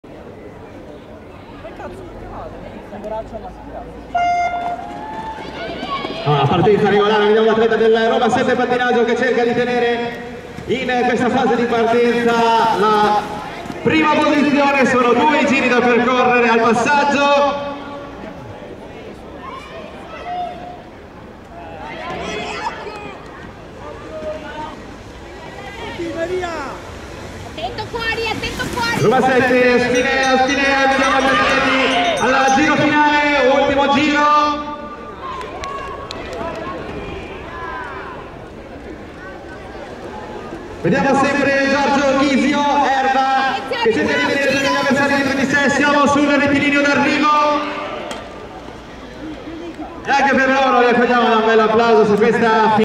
38° Memorial "A. Ponzetti" Rovigo - A/M 500m sprint, finale
38° Memorial "A. Ponzetti" - Coppa "R.Passarotto" 2024, specialità "pista".
11 Maggio 2024, Rovigo (RO) - Italy.
Organizzazione: A.S.D. Skating Club Rovigo.
Categoria Allievi Maschile.
500 metri sprint, finale.

Risultati
1  51  GHISIO ERBA GIORGIO  2885  ROMA 7 PATTINAGGIO  44.327
2  71  CAPOZZA CHRISTIAN  752  PATT.SPINEA  44.481
3  112  LEVORATO JURI  752  PATT.SPINEA  44.559
4  62  NISI ELIA  4333  R.S.VADO LIGURE ASD  44.595
5  42  SITNIC CRISTIANO  1285  PATT.S.MAURO TO  44.746
6  9  FERRARINI DANIELE  91  A.H.P. PADOVA ASD  45.635